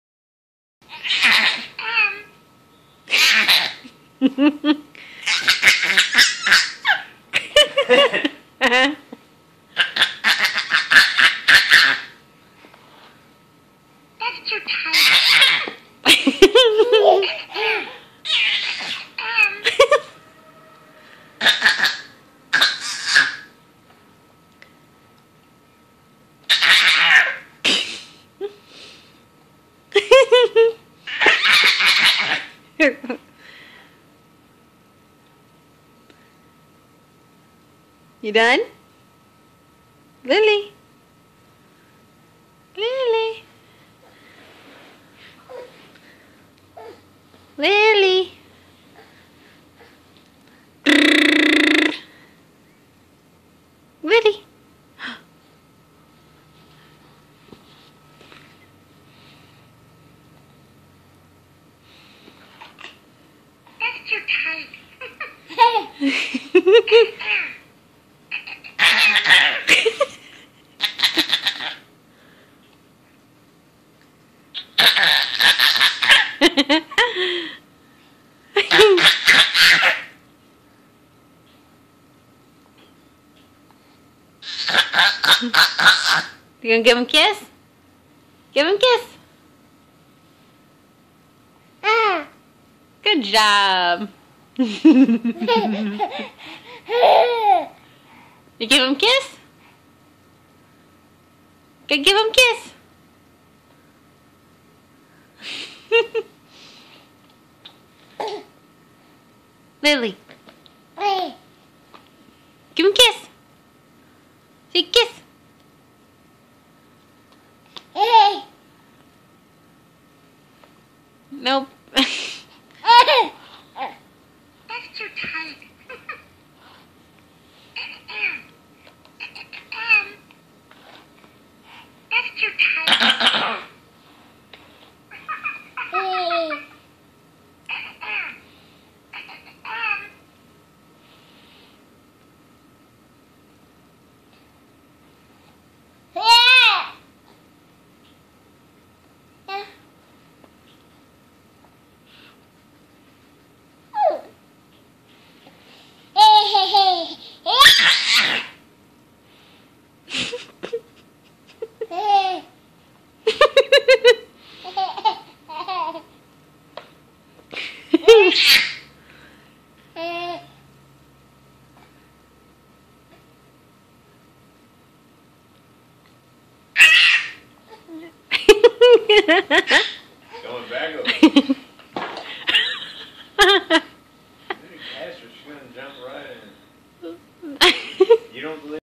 Loving Elmo
Playing with her hug me Elmo doll.